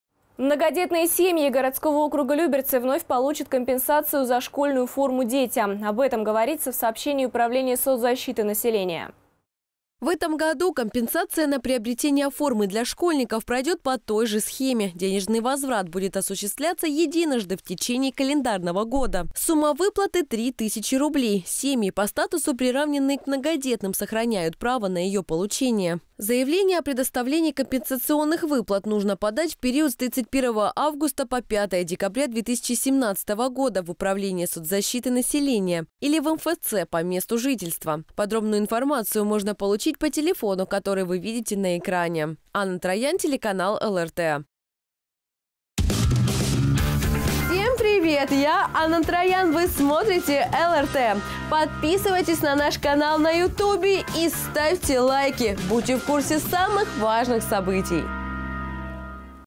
Многодетные семьи получат компенсации за школьную форму
Многодетные семьи Люберецкого округа вновь получат компенсацию на школьную форму детям. Об этом говорится в сообщении управления соцзащиты населения.

В этом году компенсация на приобретение формы для школьников пройдет по той же схеме – денежный возврат будет осуществлен единожды в течение календарного года. Сумма выплаты - 3 тысячи рублей. Семьи, по статусу приравненные к многодетным, сохраняют право на ее получение.

Заявление о предоставлении компенсационных выплат нужно подать в период с 31 августа по 5 декабря 2017 года в управление соцзащиты населения или в МФЦ по месту жительства.
Подробную информацию можно получить по телефону, который вы видите на экране.

Управление соцзащиты: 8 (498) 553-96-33

Анна Троян